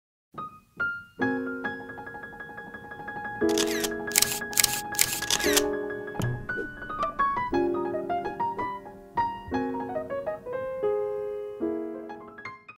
Abertura DVD - Ricardo Hara
Abertura dos vídeos da empresa Ricardo Hara FOTOGRAFIA